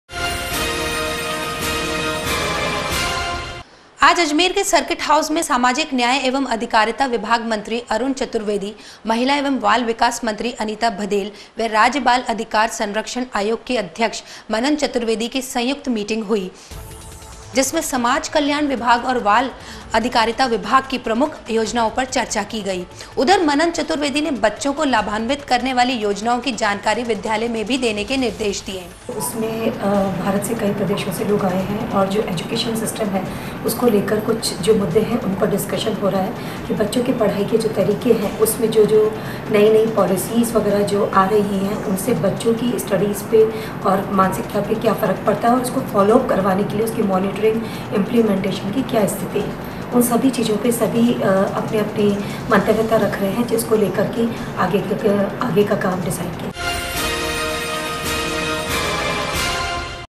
अजमेर मे समाज कल्याण विभाग की प्रमुख योजनाओं पर हुई चर्चा
आज अजमेर के सर्किट हाऊस में सामाजिक न्याय एवं अधिकारिता विभाग मंत्री अरूण चतुर्वेदी, महिला एवं बाल विकास मंत्री अनीता भदेल व राज्य बाल अधिकार संरक्षण आयोग की अध्यक्ष मनन चतुर्वेदी की संयुक्त मीटिंग हुई। जिसमें समाज कल्याण विभाग और बाल अधिकारिता विभाग की प्रमुख योजनाओं पर चर्चा की गई। उधर मनन चतुर्वेदी ने बच्चों को लाभांवित करने वाली योजनाओं की जानकारी विद्यालय में भी देने के निर्देश दिए।